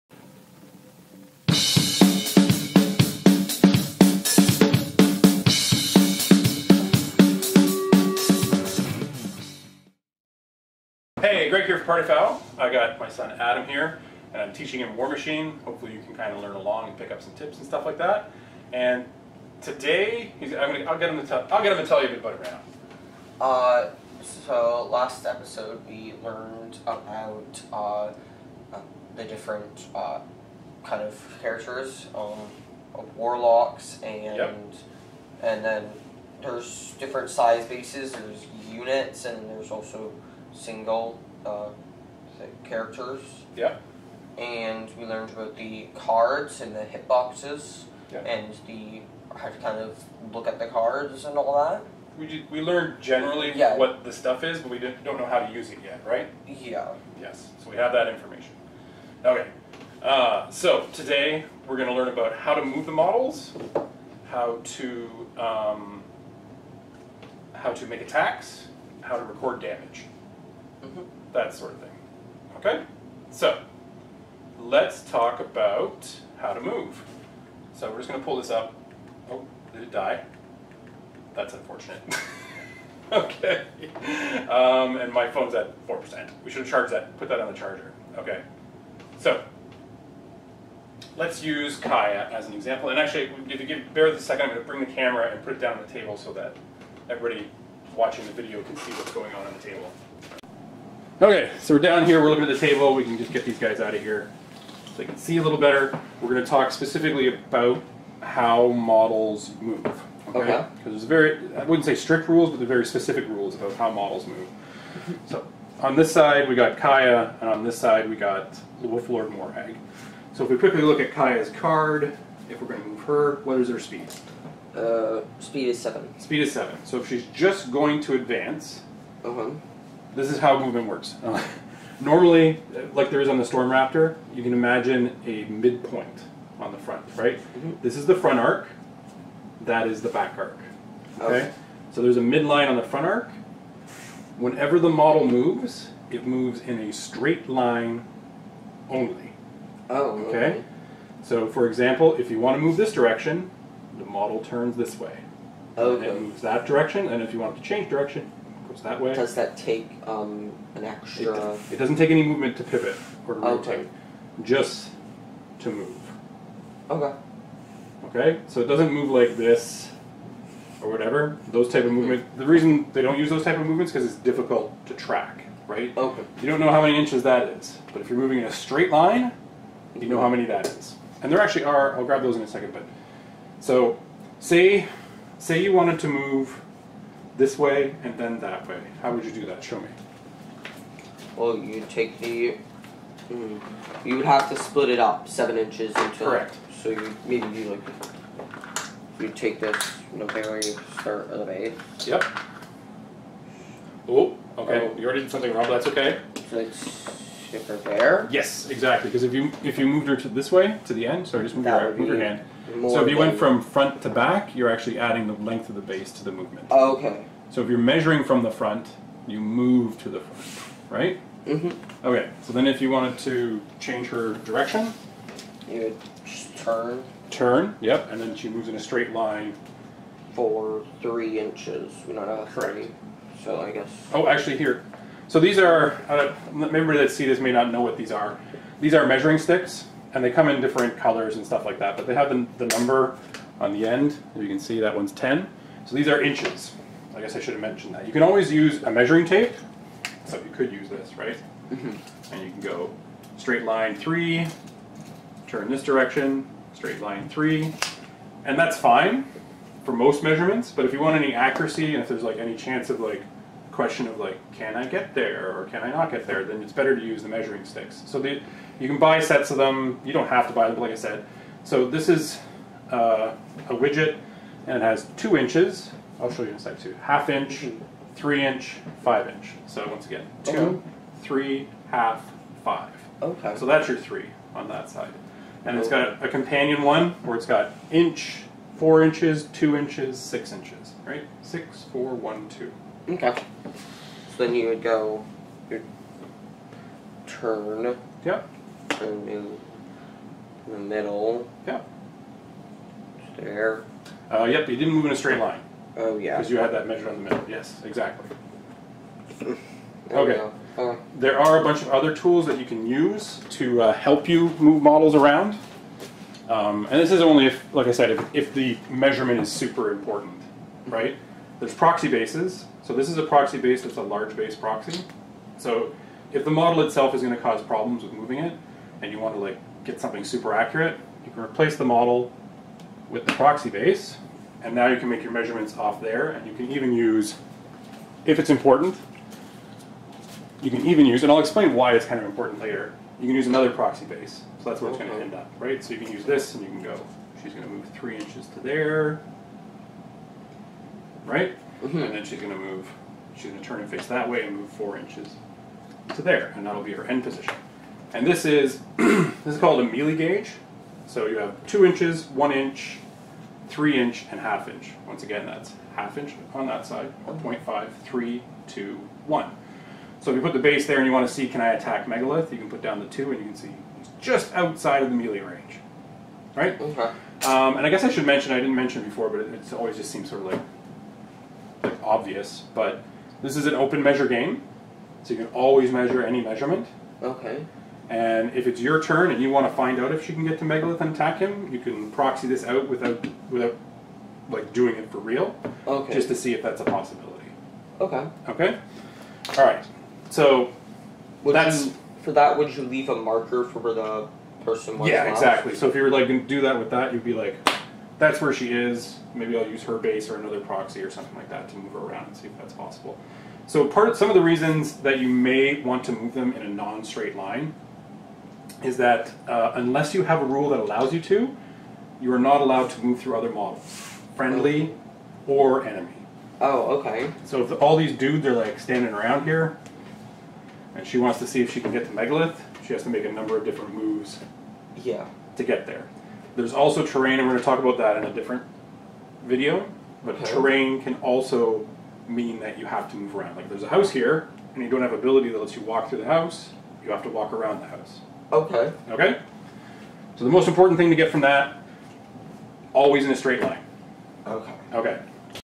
Vid 5 Moving
I'm teaching my son warmachine, watch along and learn if you're interested in playing! This video covers how to move models!